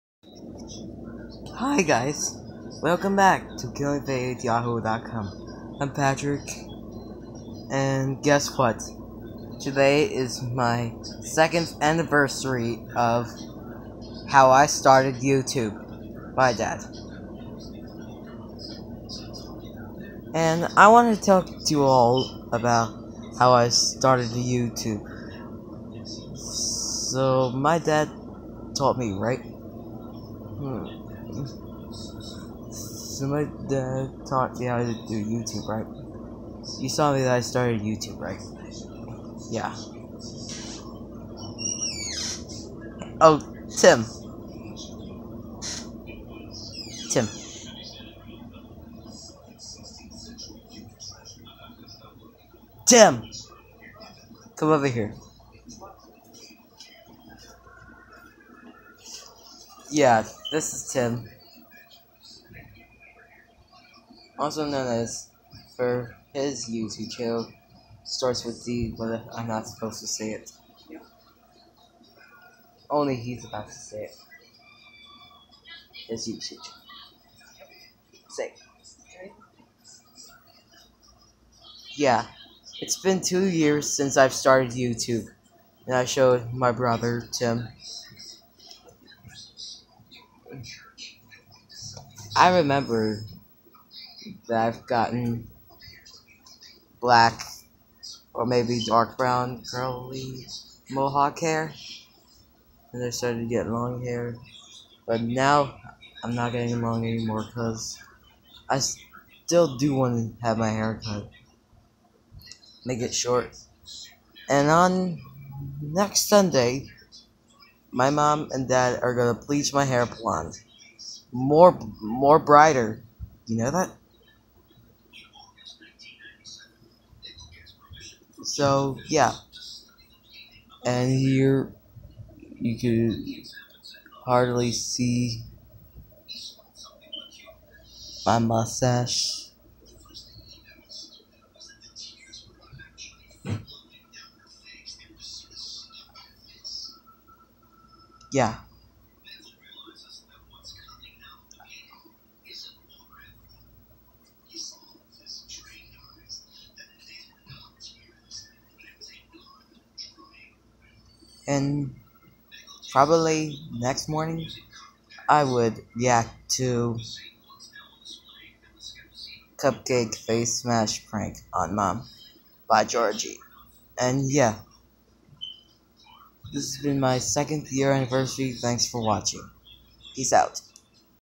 2 Years Since I Started YouTube
It's Been 2 Years Ever Since I Was 11 Years Old And I'm 13 Years Old And I'm A Teenager.